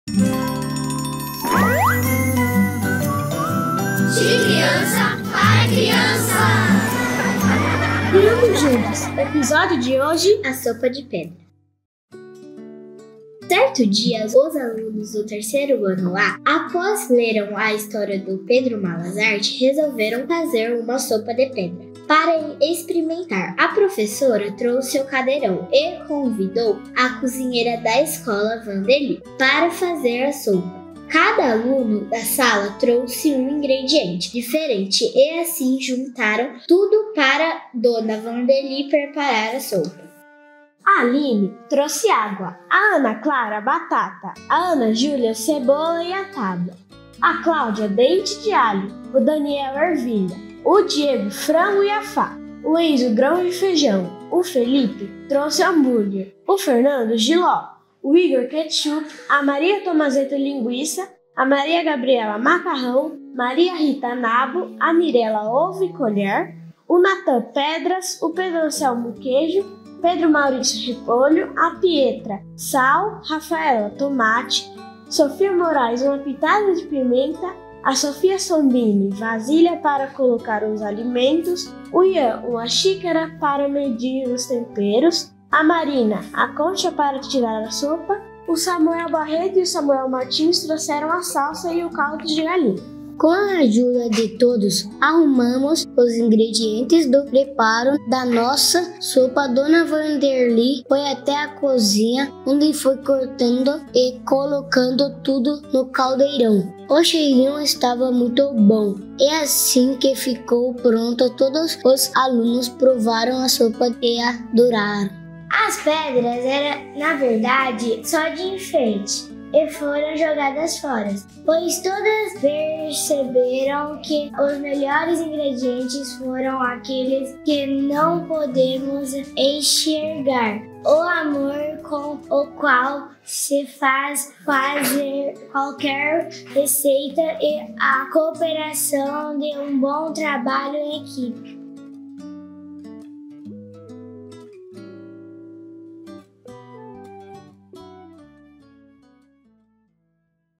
Criando Juntos 2477 | A sopa de pedras
Agradecimento à EMEB Profº Doardo Boarsari e a professora Erica Vitória dos Santos Rodrigues Nogueira pela história e desenhos enviados pela turma 3º Ano, A, Manhã (2024).